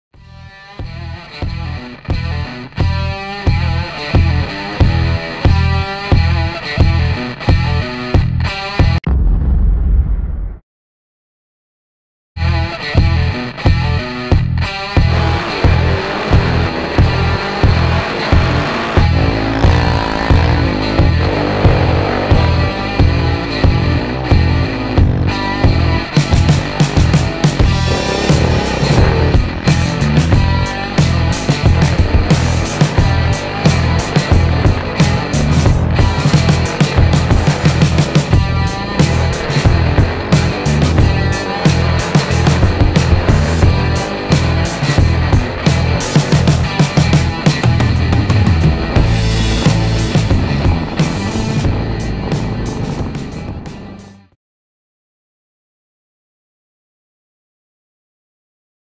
GoPro: SNOW mo RAPTOR - Raptor 700R SE with skis conversion kit and Maxxis 4-Snow Tires
First day out with my new GoPro Hero 3 Black edition camera filming 2007 Raptor 700R SE with Maxxis 4 SNOW paddle tires and custom wheel to ski conversion kit.

iMovie09 Widescreen